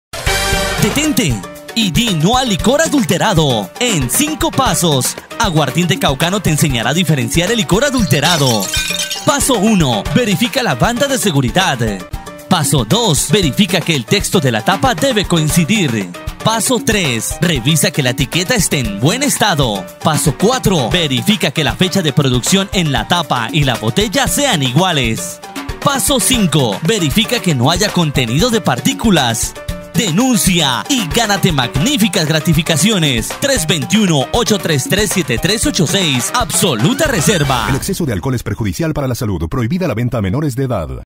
¡No juegues con tu vida di NO al licor adulterado!
¡No juegues con tu vida di NO al licor adulterado! Te enseñamos a identificar el licor adulterado en 5 Pasos. #‎LaIndustriaLicoreraDelCauca‬